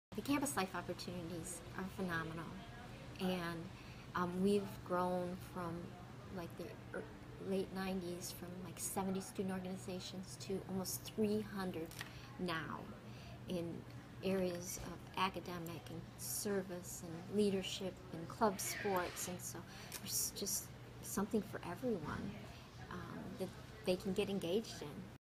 Oakland University's Jean Ann Miller on expanding student life on campus.
By: Paul Kampe - Multimedia journalist and Rochester-area beat writer for The Oakland Press/Digital First Media.